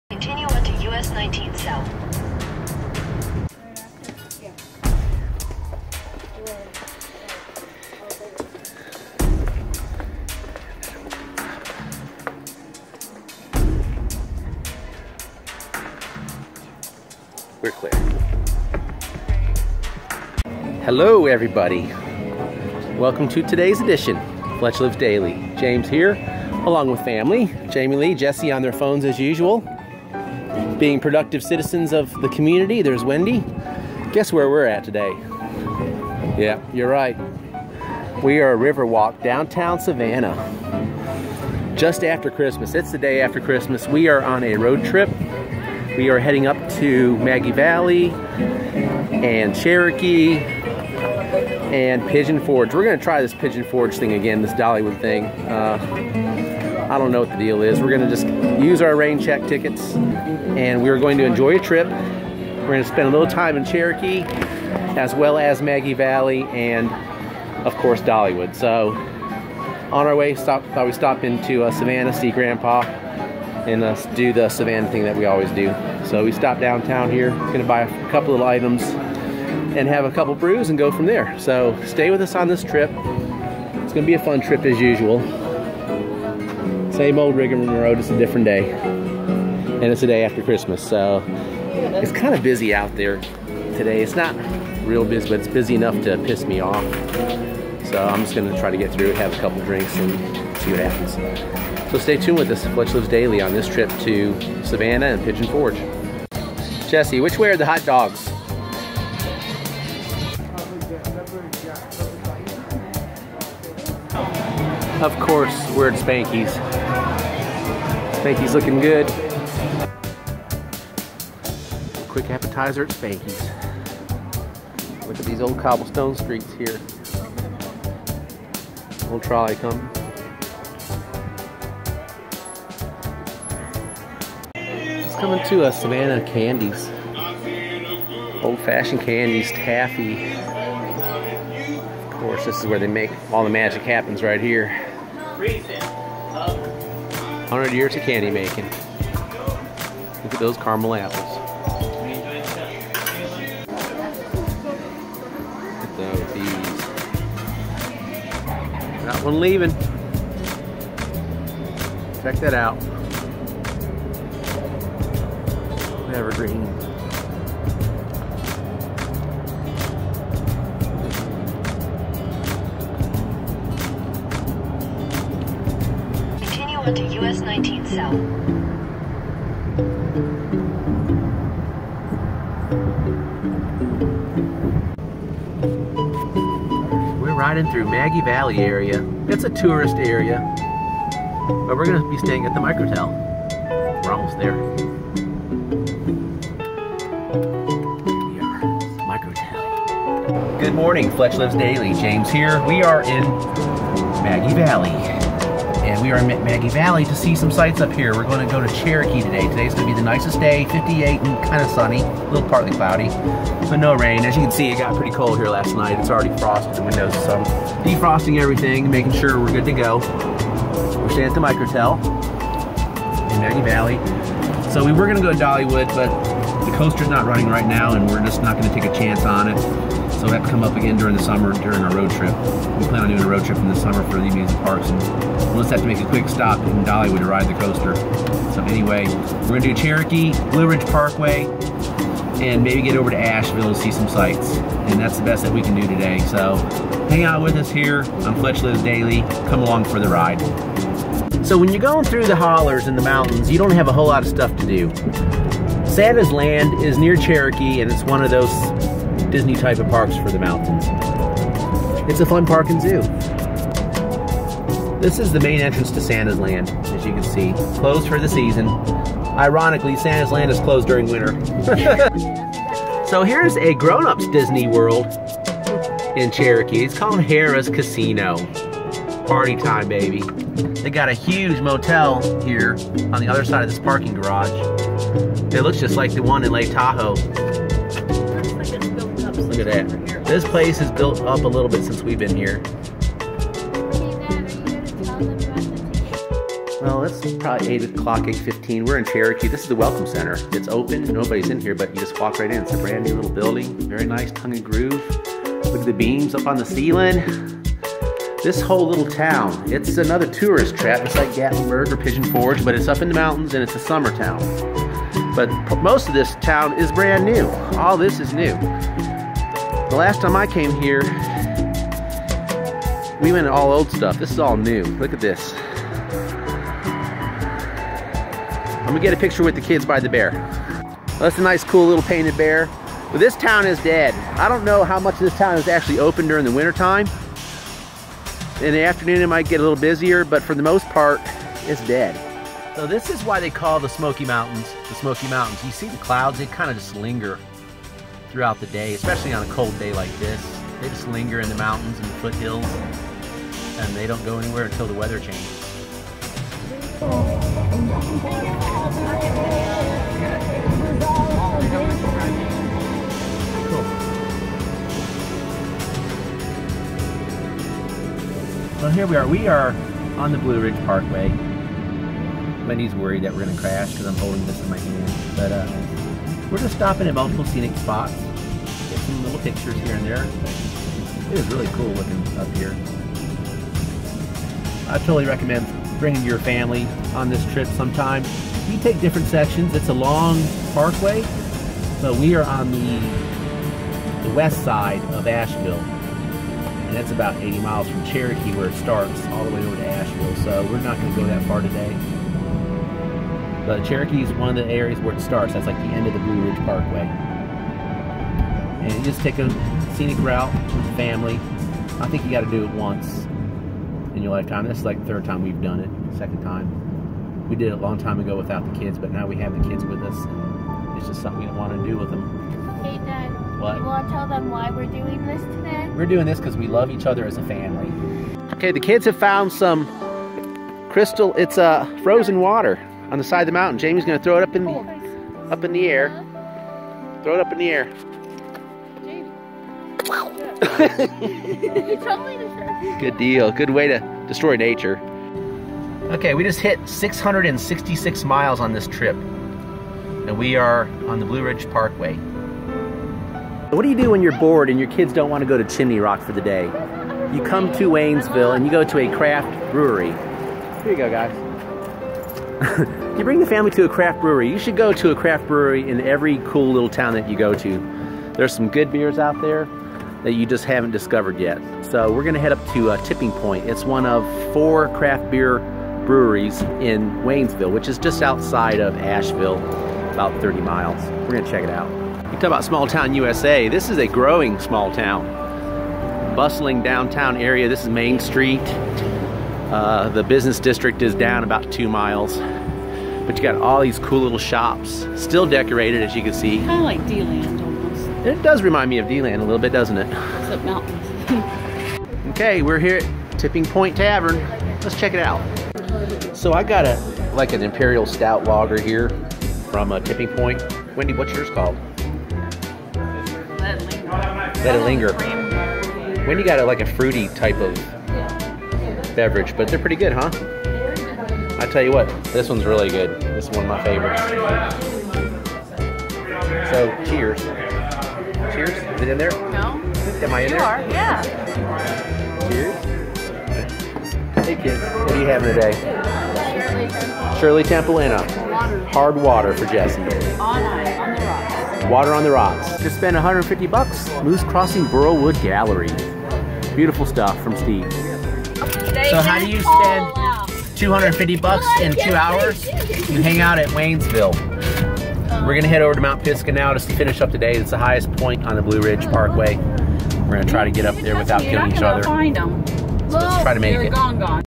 Fletch Lives Daily Goes to Savannah and Maggie Valley, Pt. 1
FLD post-xmad trip to Savannah and Maggie Valley